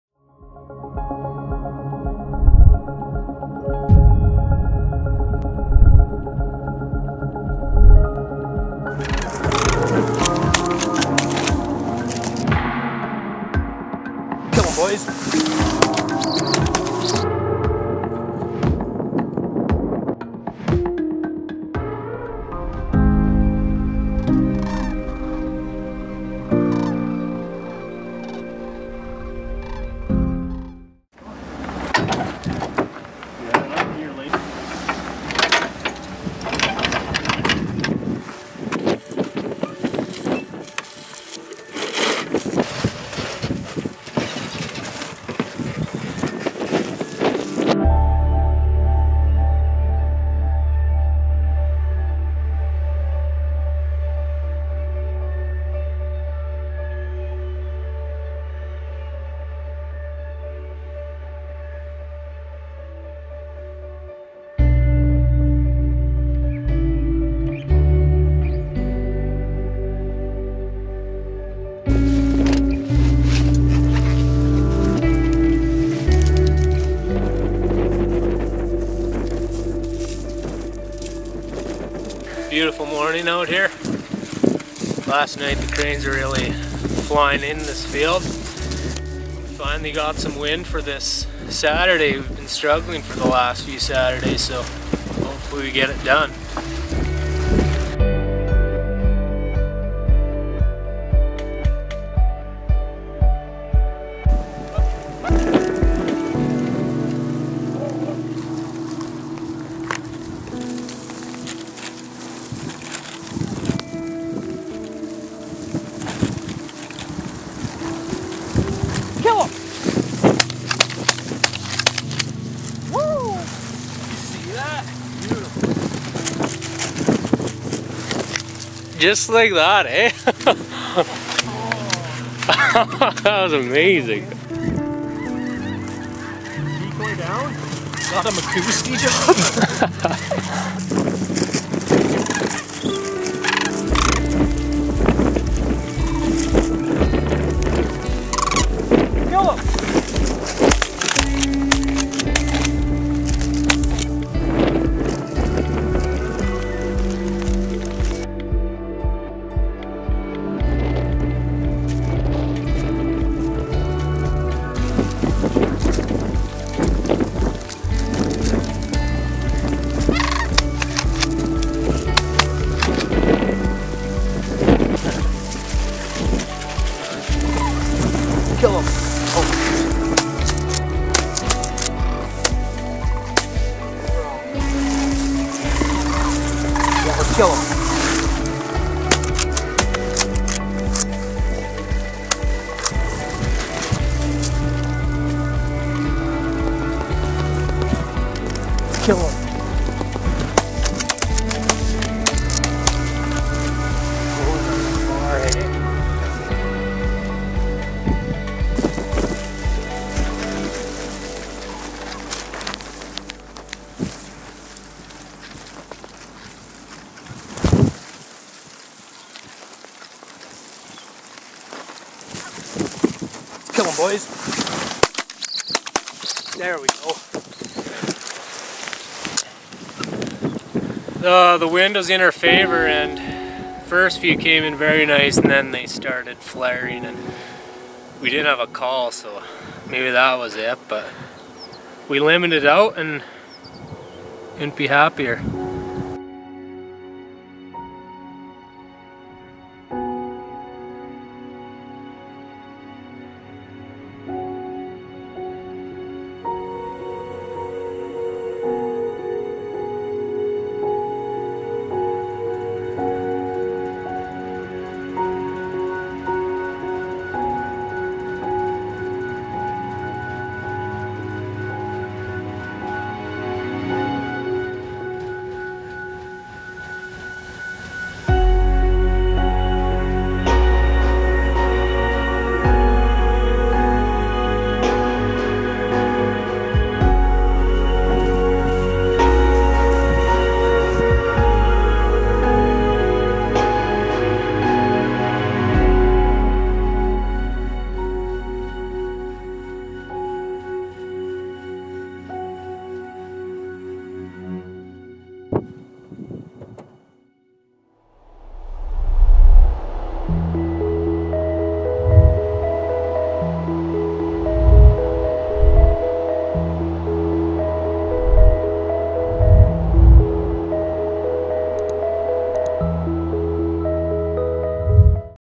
Crane Wreck | Saskatchewan Crane Hunt
October 10th 2020, a blustery day filming Saskatchewan Waterfowl go about their business limiting out with Sandhill Cranes.